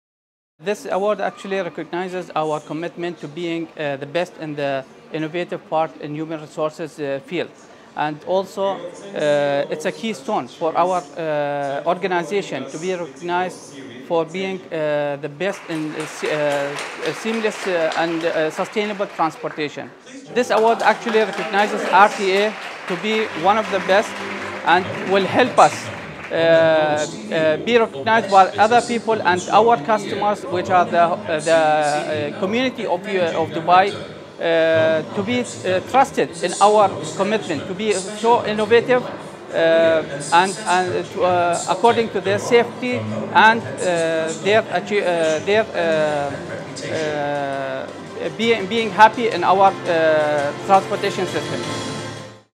RTA is a Stevie® Award winner in the 2022 Middle East & North Africa Stevie® Awards
The third annual Middle East & North Africa Stevie® Awards were presented on Saturday, 21 May at the Waldorf Astoria Hotel in Ras Al Khaimah, United Arab Emirates.